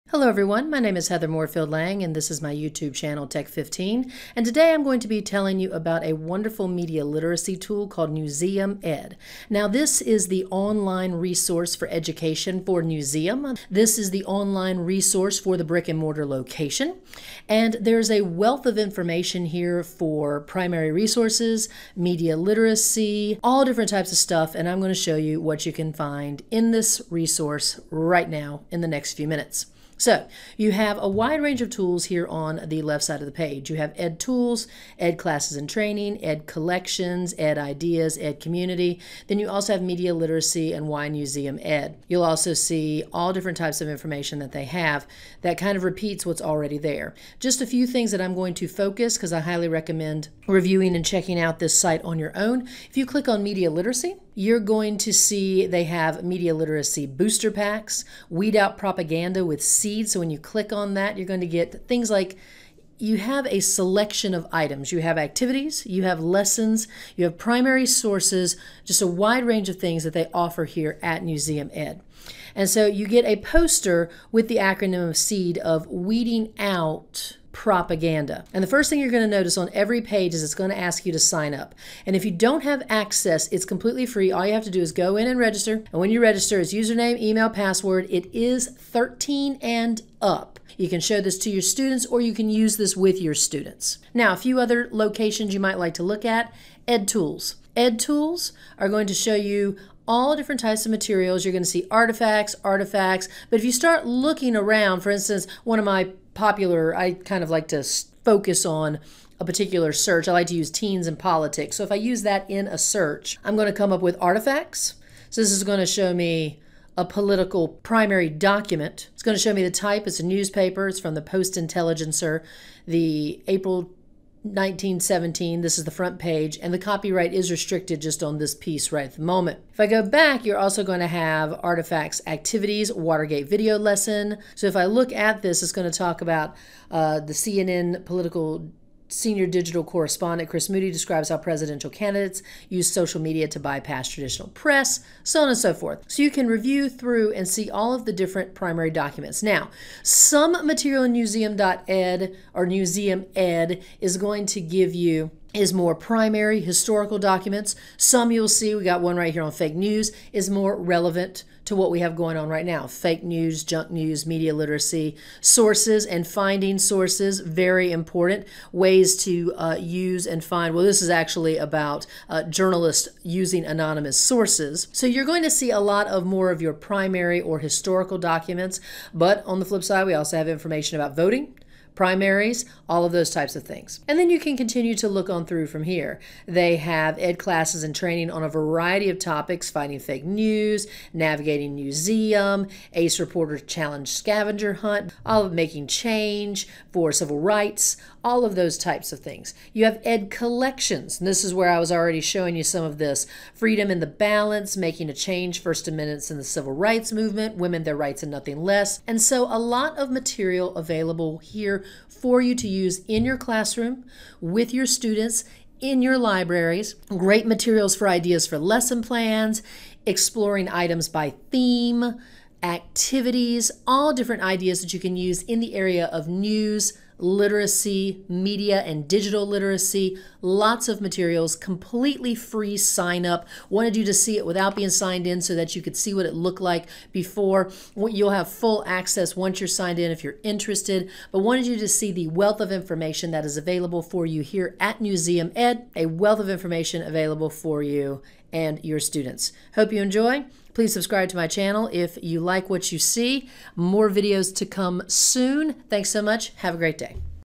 NewseumEd: News and Education
Learn about Newsuem Ed and all its great resources in this short video.